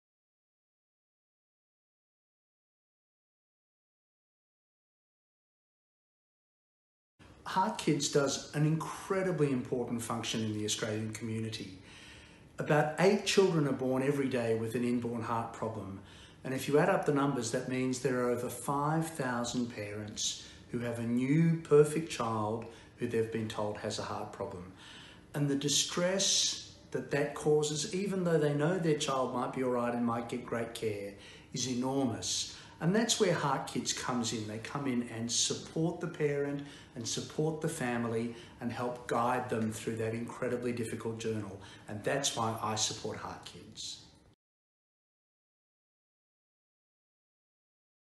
Prof David Celermajer Why I support HeartKids
Professor David Celermajer, Professor of Cardiology at the University of Sydney, talks about the important role that HeartKids plays in supporting those families living with the impact of congenital heart disease.